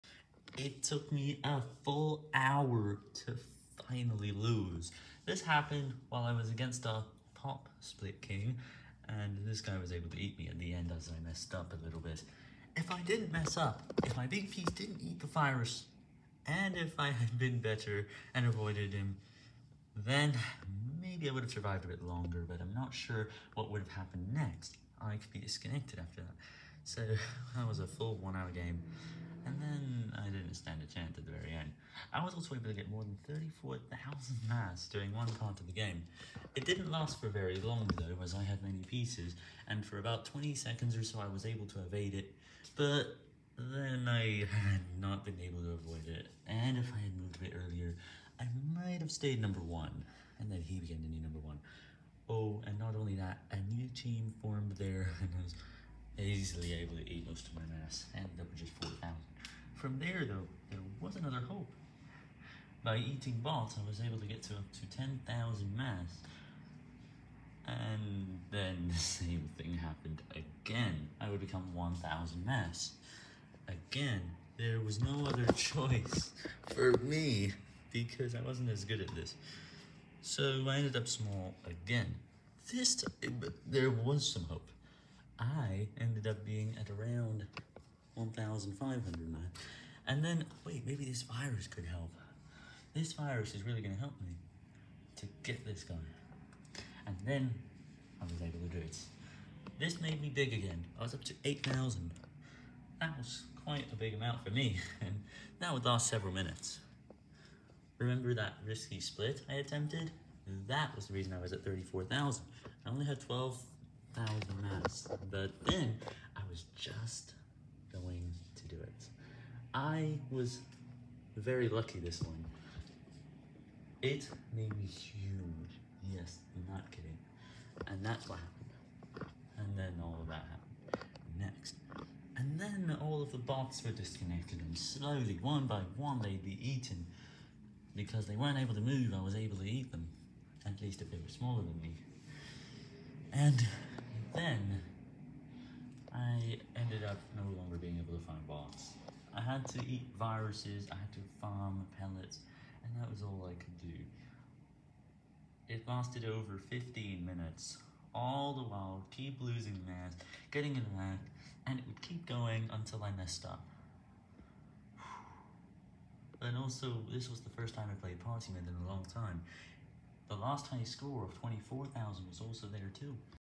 This game lasted one hour.
My game lasted an hour before I messed up. But I tell you what happened in between.